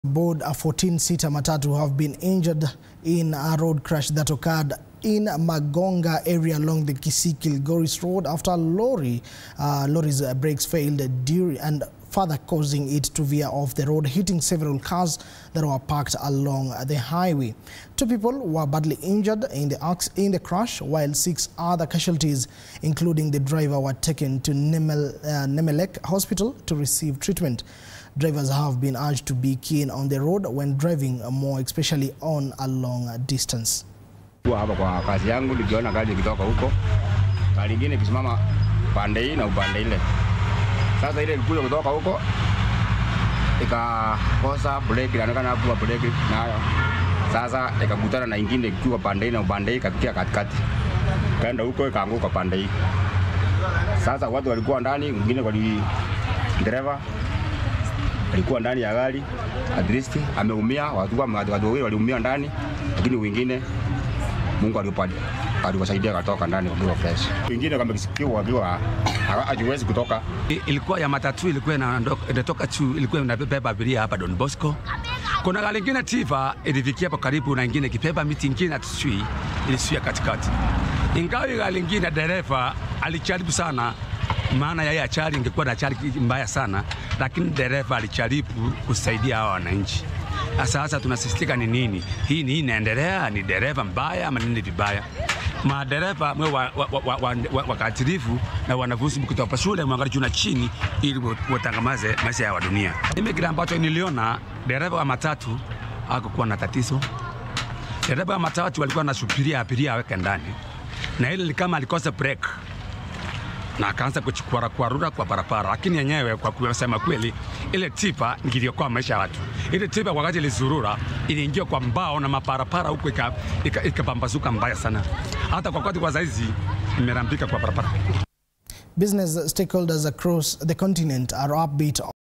Several passengers have been badly injured along the Kisii-Kilgoris road
Several passengers aboard a 14 seater matatu have  been injured  badly in an accident that occurred  in Mogonga area along the Kisii -Kilgoris road after a lorry’s breaks failed causing it to veer off the road hitting several cars that were packed along the highway.